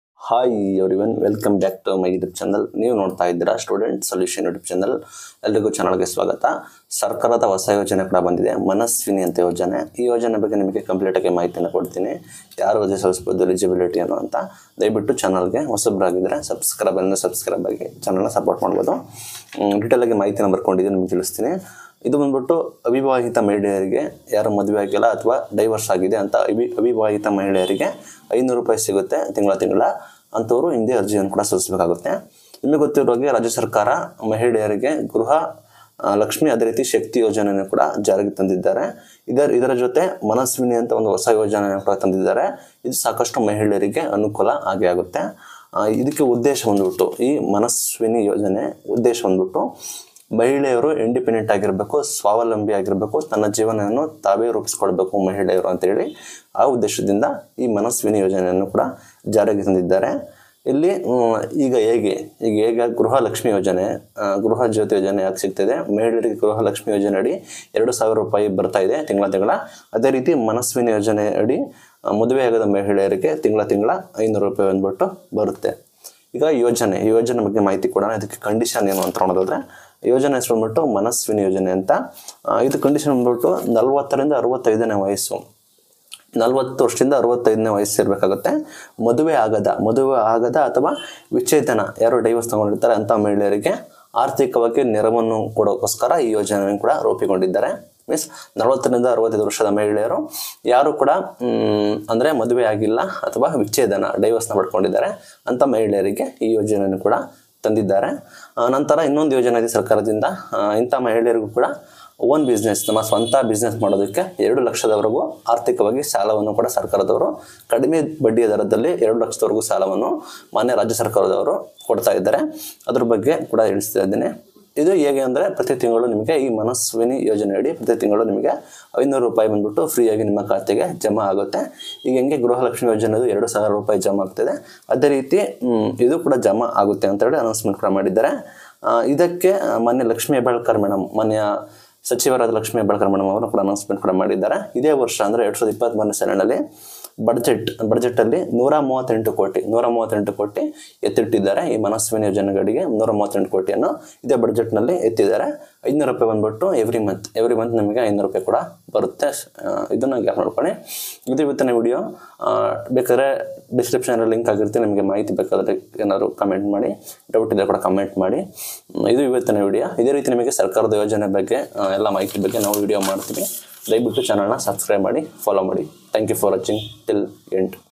ಮನಸ್ವಿನಿ ಯೋಜನೆ|ಮಹಿಳೆಯರಿಗೆ ಉಚಿತವಾಗಿ 500 ರೂಪಾಯಿ ಪ್ರತಿ ತಿಂಗಳು ನೆರವಾಗಿ ಖಾತೆಗೆ||students solution
Upi id:hatersvtu@ybl